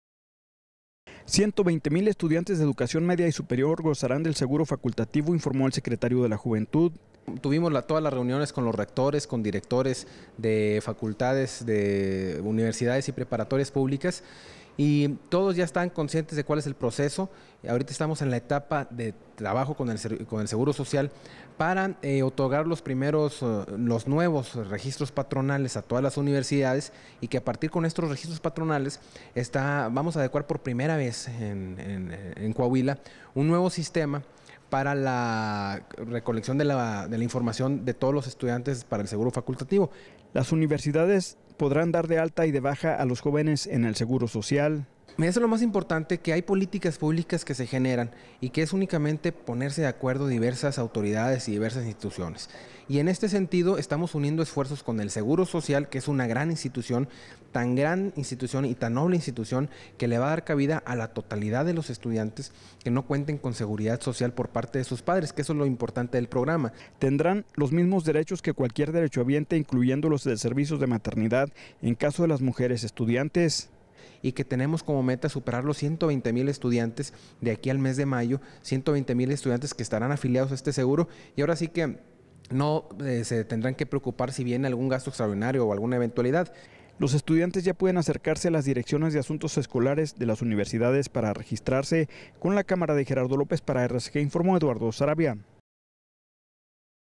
En mayo empezará a funcionar el Seguro Facultativo
Ciento veinte mil estudiantes de educación media y superior gozarán del seguro facultativo informó el Secretario de la Juventud, Carlos García Vega.